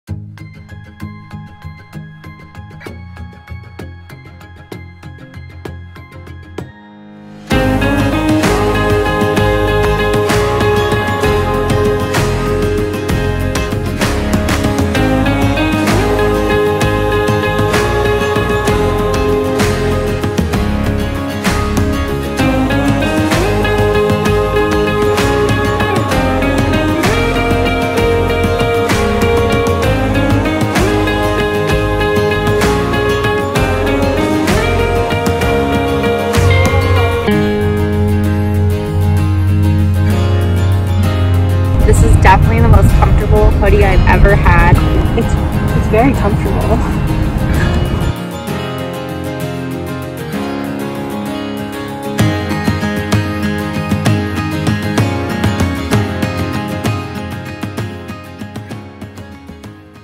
Ultimate Travel Hoodie for Unparalleled Comfort Anywhere
Comfiest travel hoodie ever with memory foam padding for your comfort !